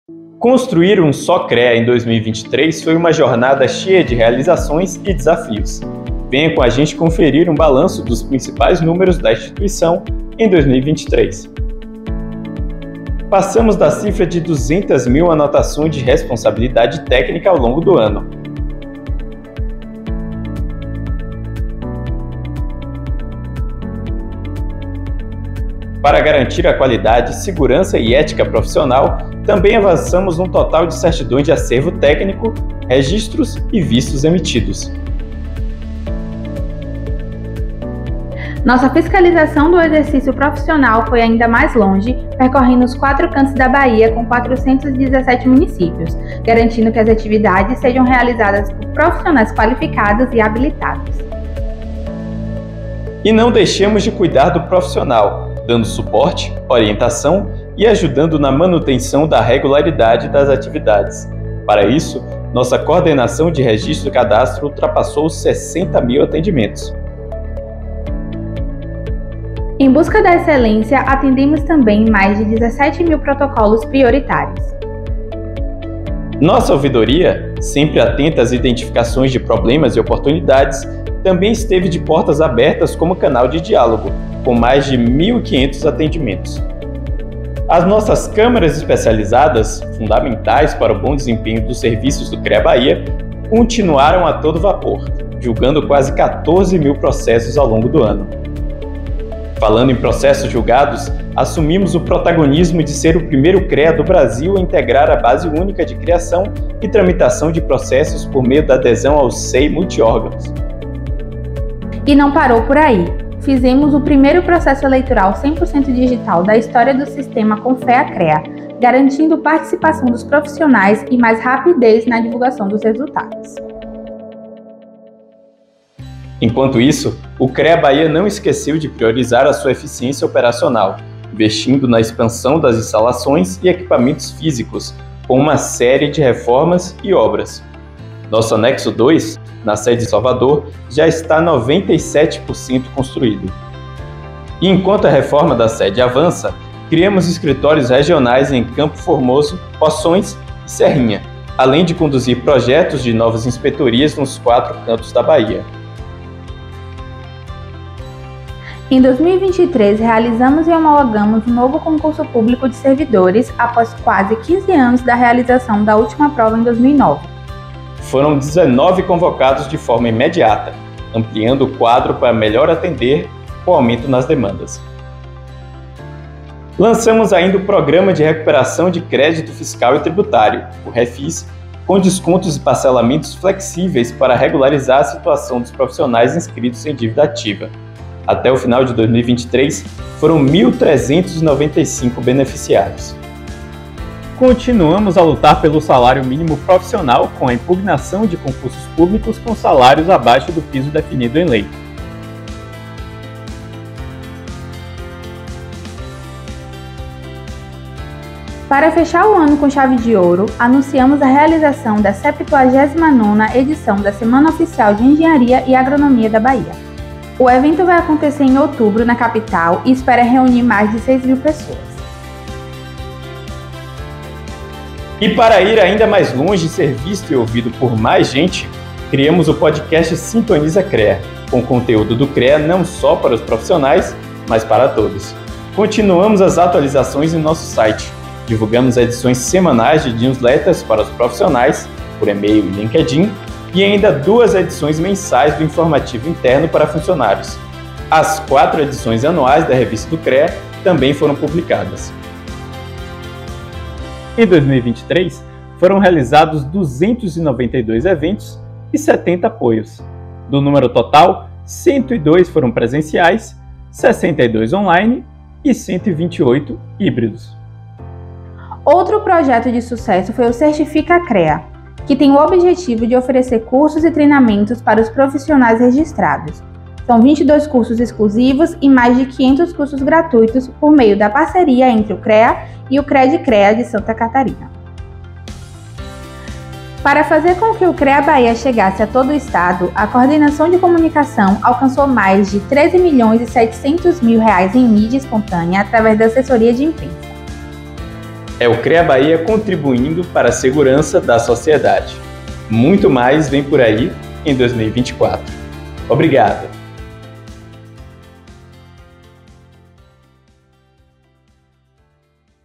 Crea-BA em Números - 2023
Quantas ações de fiscalização foram necessárias para fazer UmSóCrea em 2023? E quantos projetos implementados, obras iniciadas, inovações e avanços nos levaram mais longe? Com certeza, a resposta vai muito além da soma dos números. Confira esse e outros destaques no balanço da instituição, que mostra um panorama das diversas frentes de atuação do Conselho durante o ano passado.